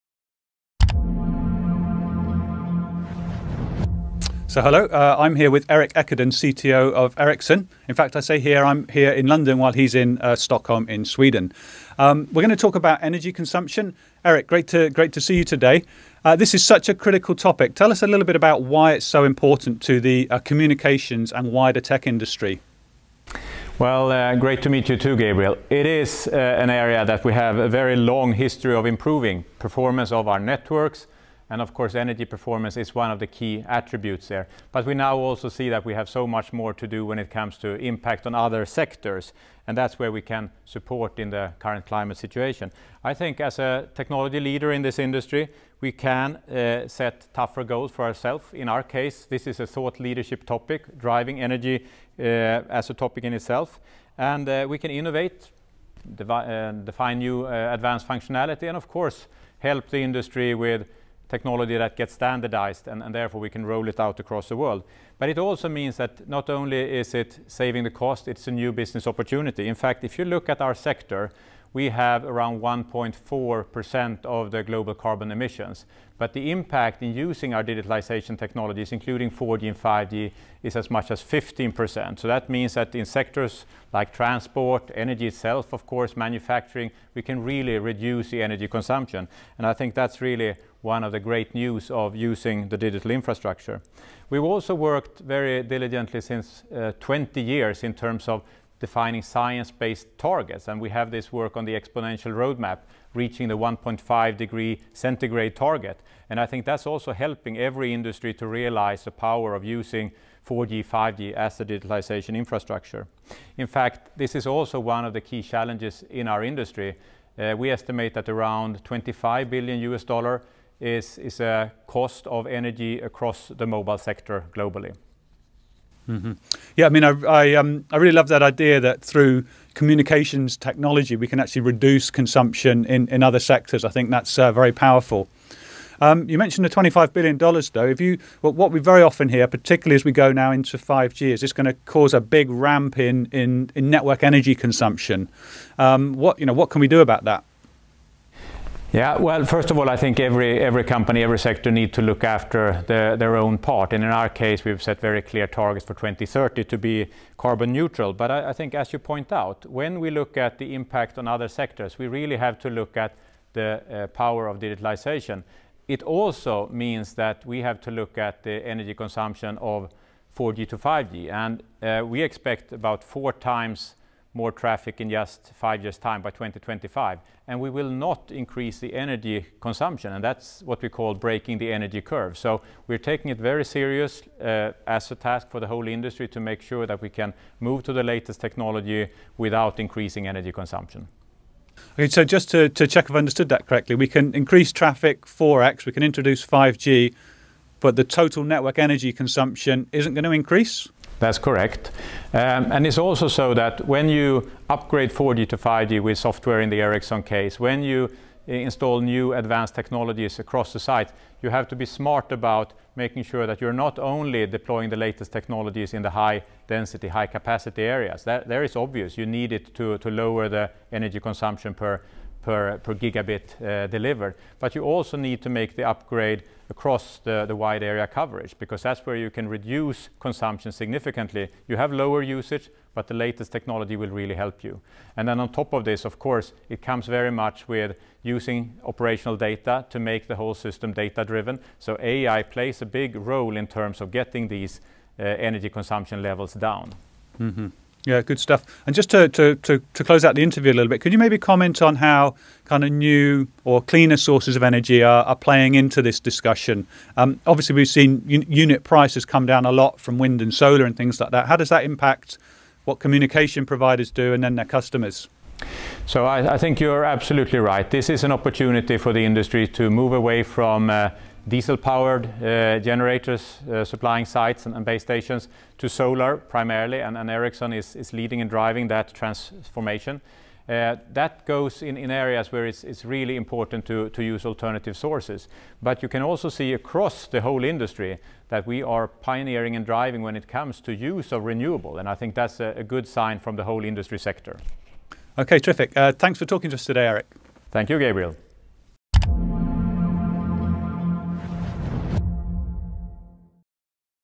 Breaking the energy curve of mobile networks
Erik Ekudden, CTO and Head of Technology & Strategy at Ericsson, explains how Ericsson's solutions allow operators to achieve significant energy savings all whilst managing expected growth in data traffic, meeting the needs of both current and ...